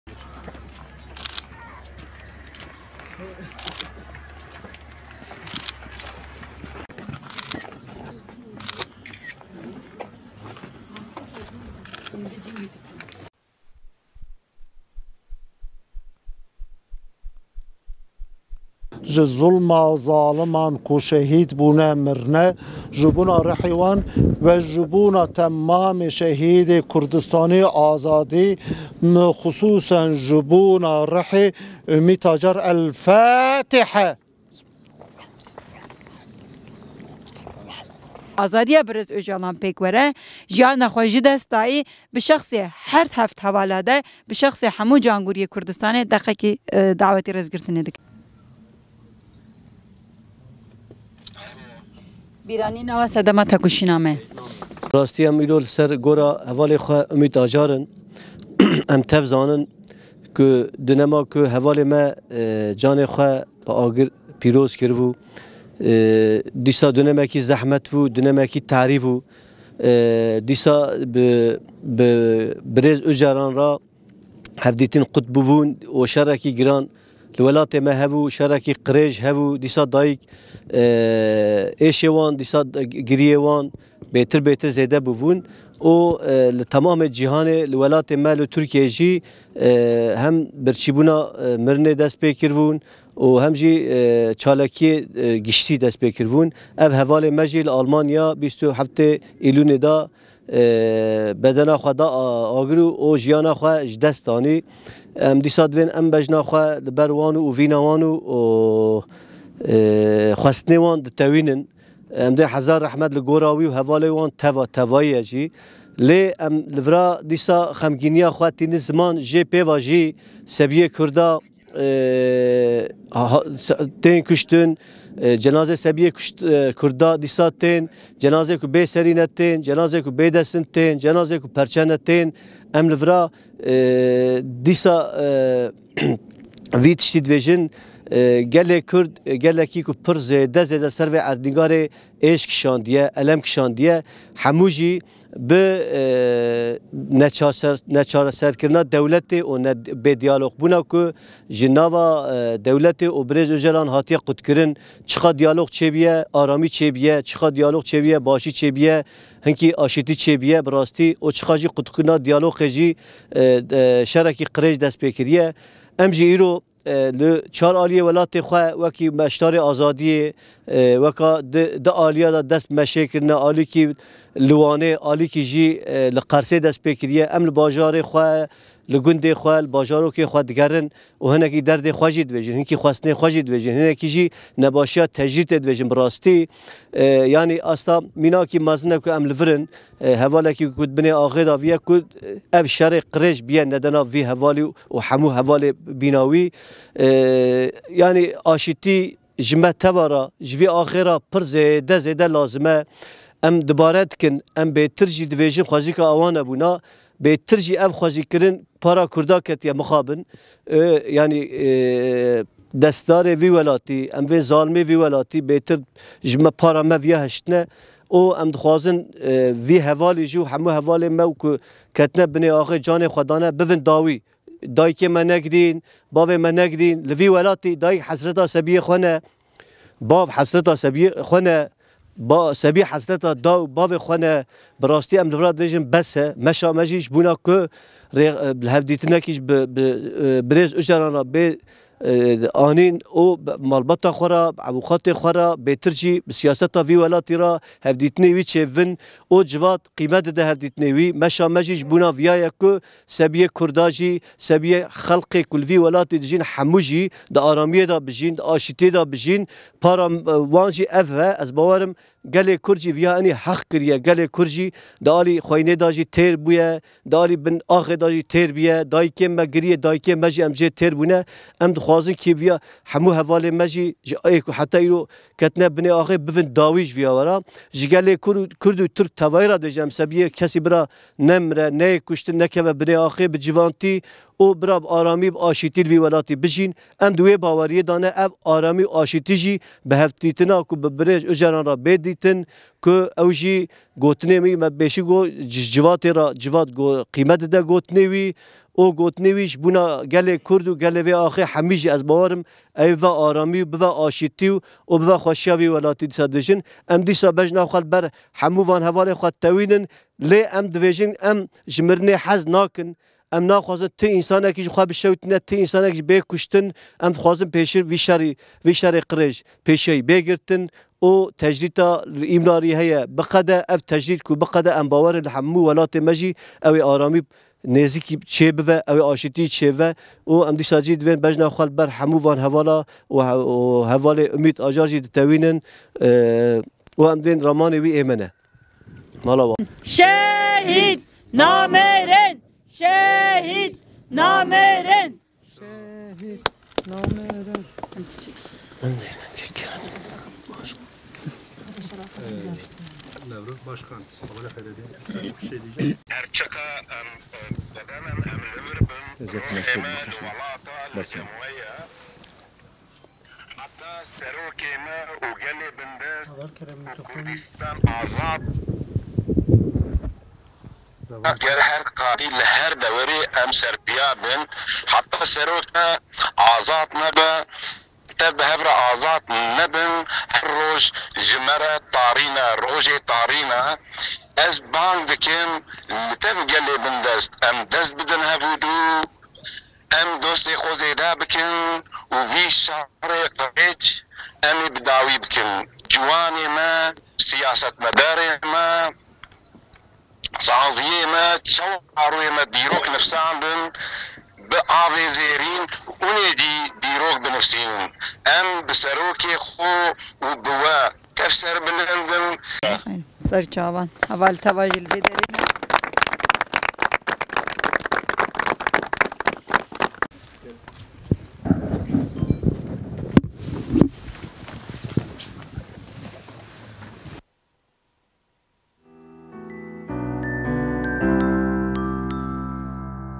ÖZGÜRLÜK YÜRÜYÜŞÇÜLERİ ÜMİT ACAR'I MEZARI BAŞINDA ANDI
PKK Lideri Abdullah Öcalan’ın fiziki özgürlüğü için yürüyen “Büyük Özgürlük Yürüyüşü”nün Qers kolu, 2018’de tecride karşı bedenini ateşe veren Ümit Acar'ı mezarı başında andı.